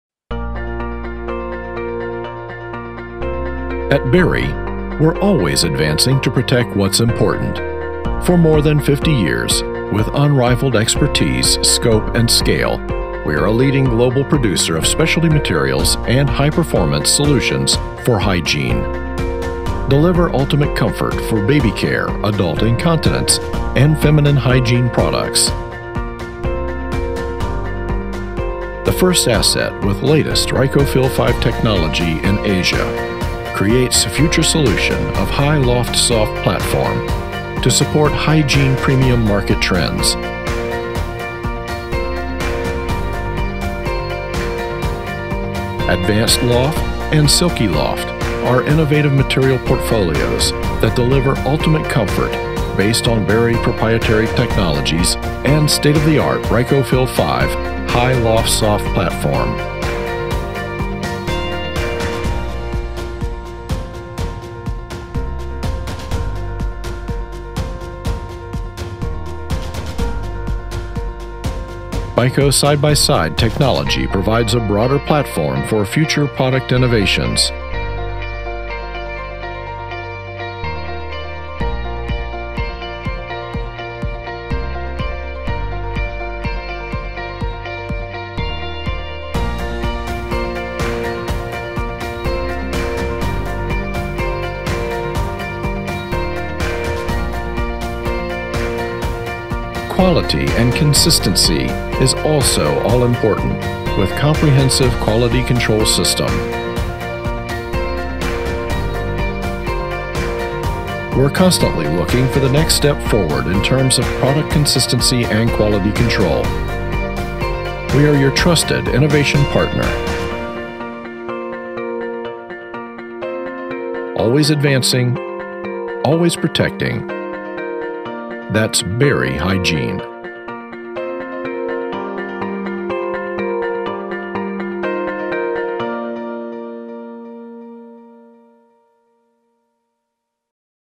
Differentiated Solutions through R5 Technology
The third R5 start-up in the world, discover how our investment in a state of the art Reicofil R5 asset supports the latest innovations of the global hygiene market.